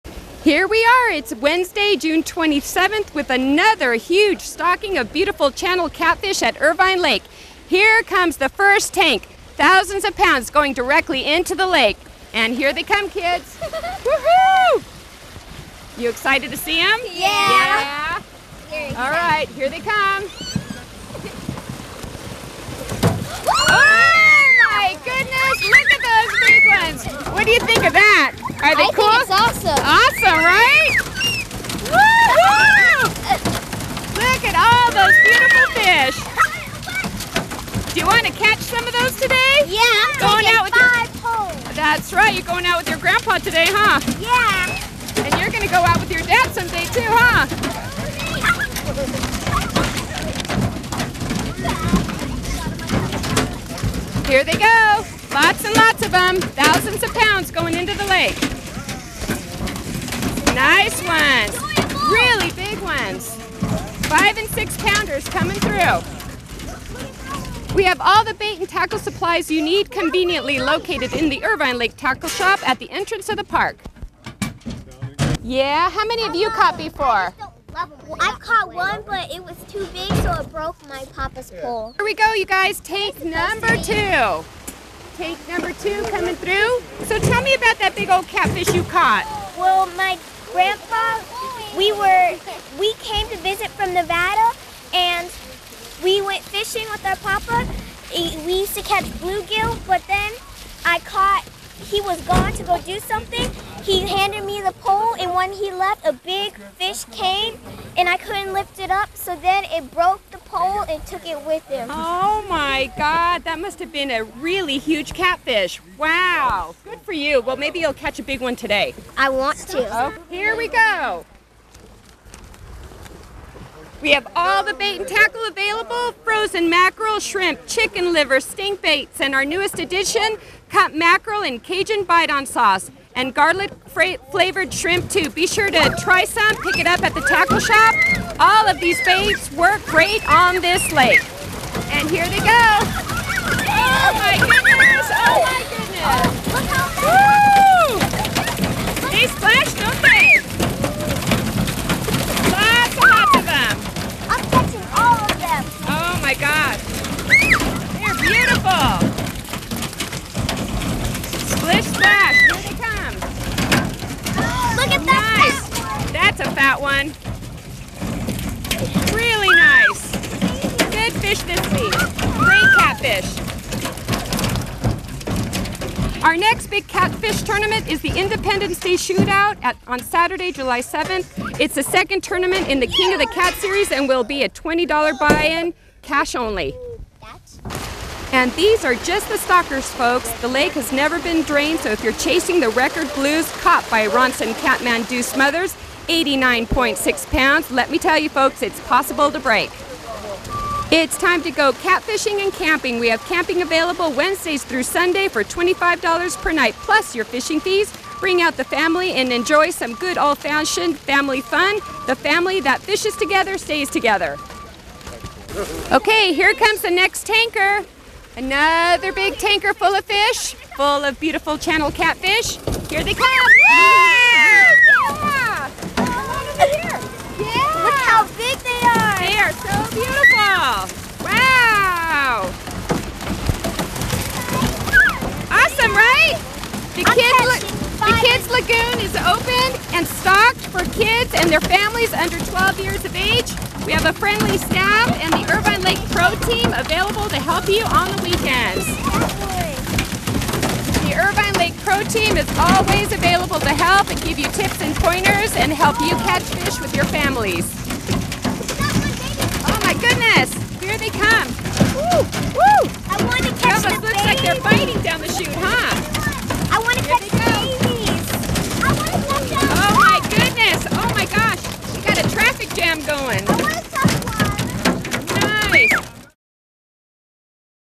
June 27 Catfish Stocking - Irvine Lake
Huge stocking of Catfish, Wednesday, June 27, at Irvine Lake.  Check it out !!